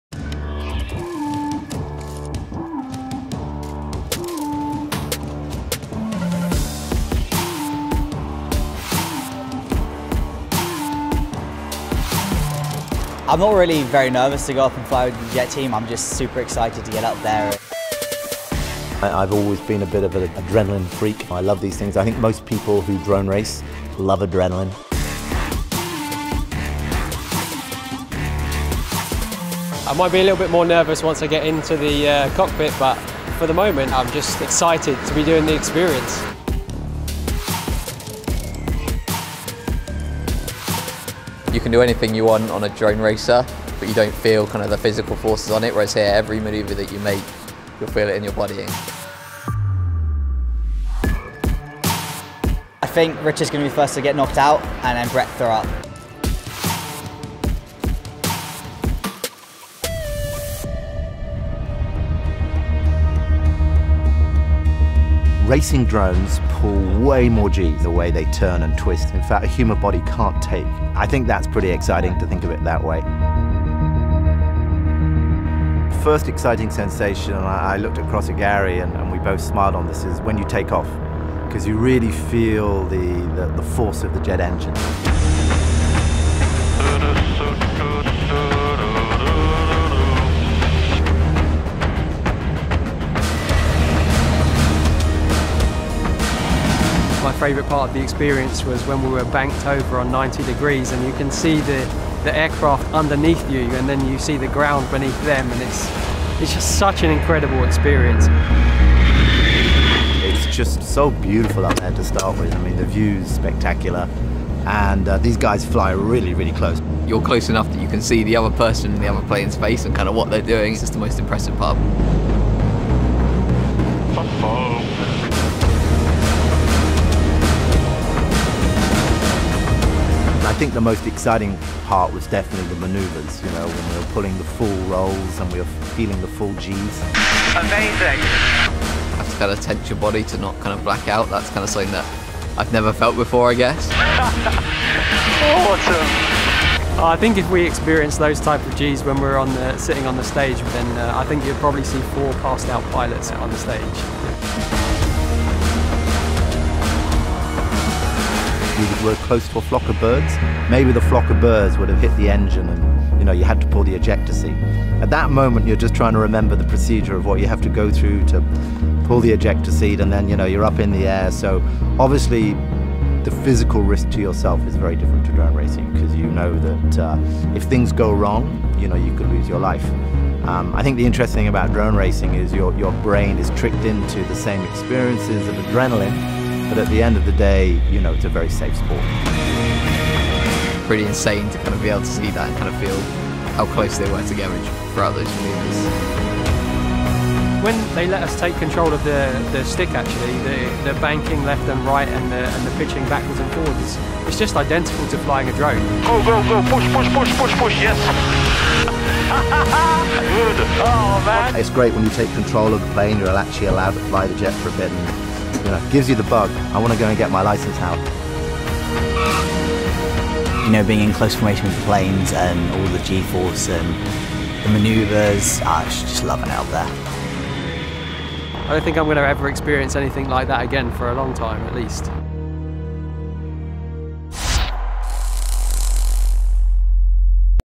XBlades Racing & The Breitling Jet Team
The adrenaline rush reaches new heights when XBlades Racing founder and CEO Richard de Aragues, and pilots Luke Bannister (BanniUK) Gary Kent (JusticeFPV) and Brett Collis (COLLISionFPV) swap their FPV goggles for the cockpit of an incredible Breitling jet! The Breitling Jet Team gave XBlades the amazing and rare opportunity to take to the skies in these beautiful flying beasts, even letting our CEO and pilots take control of the stick for a while allowing them to compare the handling of a jet to that of a racing quad! Thank you Breitling and DCL for a mind blowing experience!